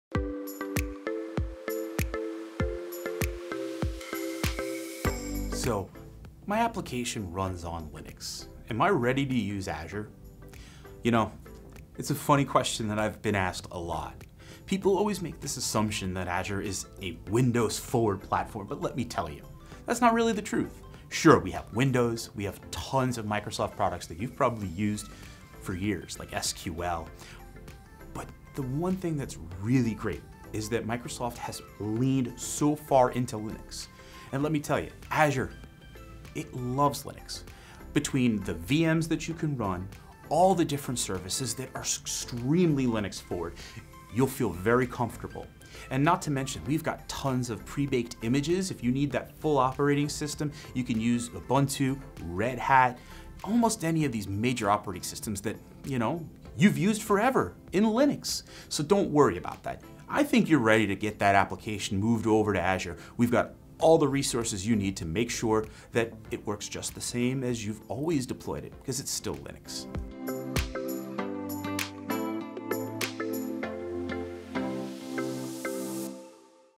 My application runs on Linux, am I ready to use Azure? | One Ops Question
In this short video Jay explains how you can run your Linux based application on Azure.

Create a free Azure Account today and give it a try